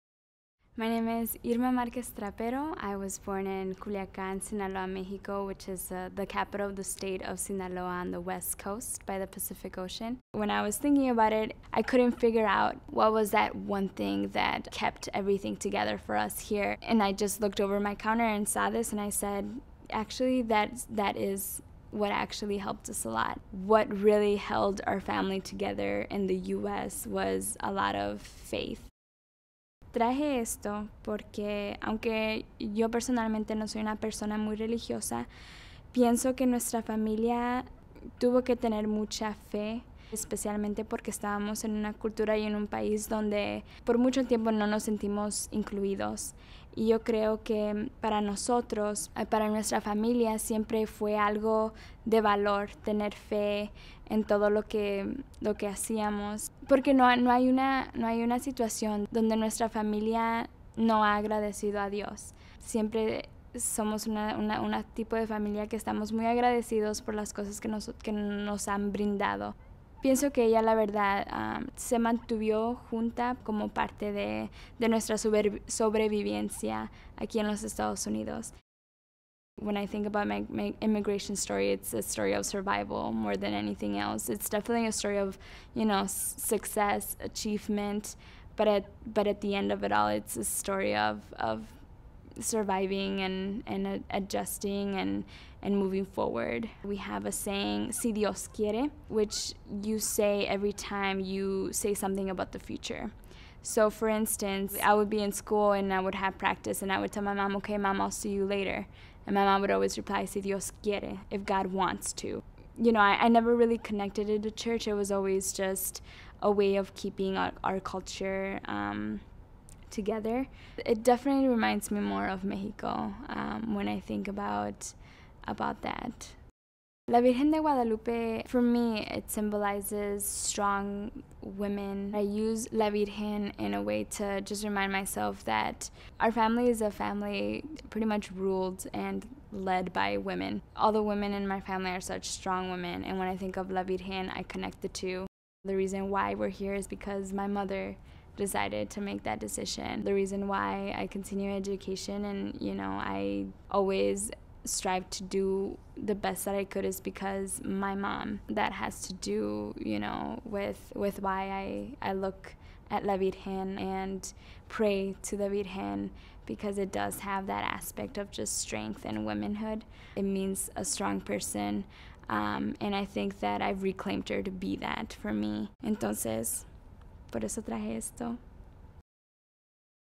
Irma Marquez Trapero: Immigrant Stories
Irma explains how faith, symbolized by a candle of la Virgen de Guadalupe, gave her family strength during their migration from Mexico and adjustment to life in the United States.

immigrantstories.umn.edu